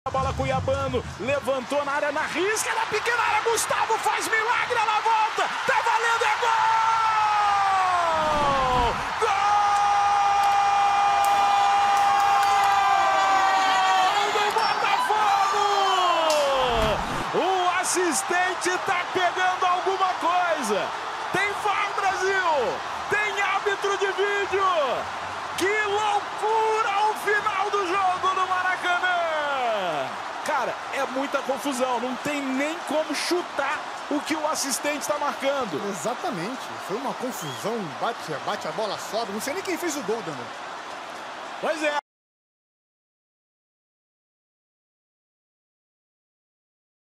LANCE DO GOL ANULADO DO BOTAFOGO CONTRA O CRICIUMA | 18/10/24
Lance polemico no gol anulado do Botafogo vs Criciuma | 18/10/24

Se for utilizar meus vídeos em seus projetos, por favor, deixe os devidos créditos.
If you use my videos on your projects, please leave credits.